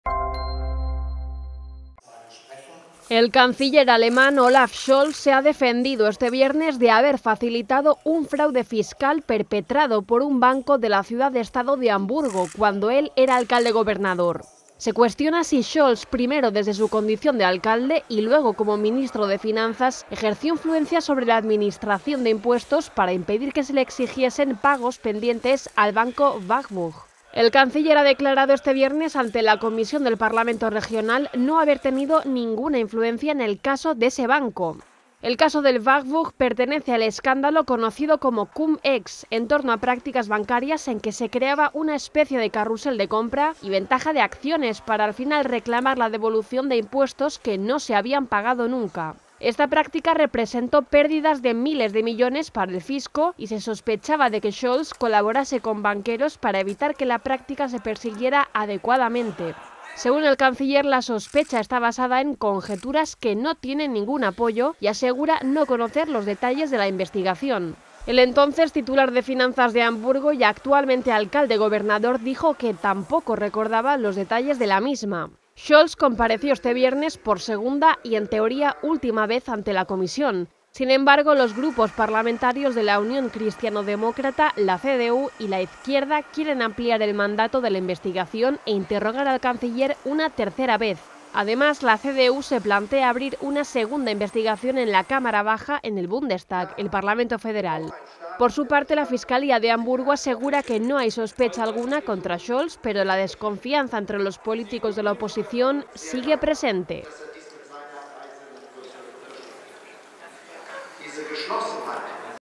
Scholz se defiende de acusación de facilitar fraude fiscal
Hamburgo (Alemania), 19 ago (EFE/EPA).- (Imagen: Clemens Bilan) El canciller alemán, Olaf Scholz, se defendió este viernes de haber facilitado un fraude fiscal perpetrado por un banco de la ciudad estado de Hamburgo cuando él era alcalde gobernador, ante una comisión investigadora en el parlamento de la ciudad.

Edición y locución: Claudia Escandell